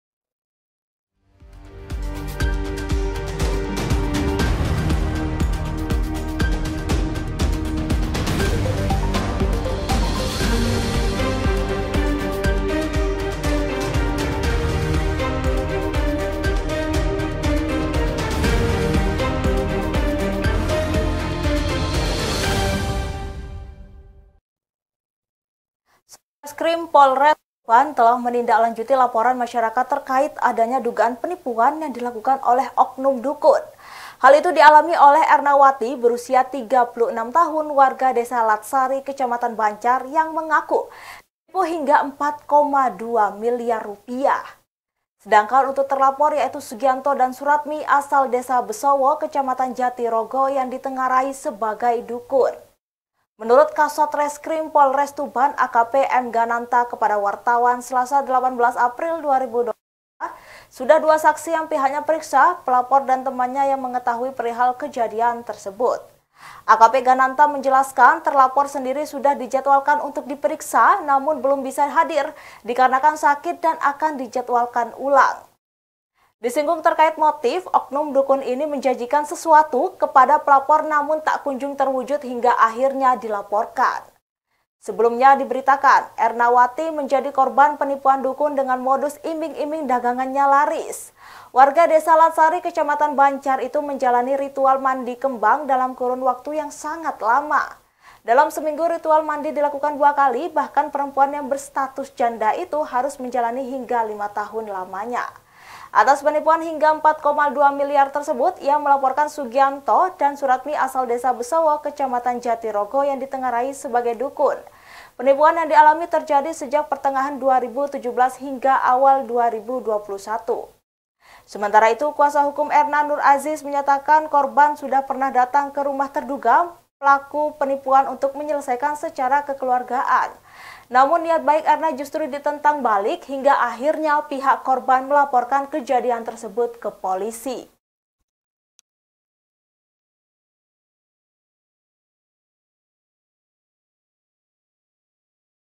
Mbah Dukun Terlapor Penipuan Rp 4,2 M Batal Diperiksa, Polisi: Alasan Sakit
TUBAN - Satreskrim Polres Tuban telah menindaklanjuti laporan masyarakat terkait adanya dugaan penipuan yang dilakukan oleh oknum dukun. Hal itu dialami Ernawati (36), warga Desa Latsari, Kecamatan Bancar, yang mengaku ditipu hingga Rp 4,2 miliar.

Sedangkan untuk terlapor yaitu Sugianto dan Suratmi asal Desa Besowo, Kecamatan Jatirogo, yang ditengarai sebagai dukun. "Sudah dua saksi yang kita periksa, pelapor dan temannya yang mengetahui perihal kejadian tersebut," kata Kasat Reskrim Polres Tuban, AKP M Gananta kepada wartawan, Selasa (18/4/2023).

Perwira pertama itu menjelaskan, terlapor sendiri sudah dijadwalkan untuk diperiksa namun belum bisa hadir. Disinggung terkait motif, oknum dukun ini menjanjikan sesuatu kepada pelapor namun tak kunjung terwujud hingga akhirnya dilaporkan.

"Tidak hadir pada pemeriksaan pertama alasannya sakit, akan kita jadwalkan ulang," pungkas Gananta. Sebelumnya diberitakan, Ernawati (36), menjadi korban penipuan dukun dengan modus iming-iming dagangannya laris.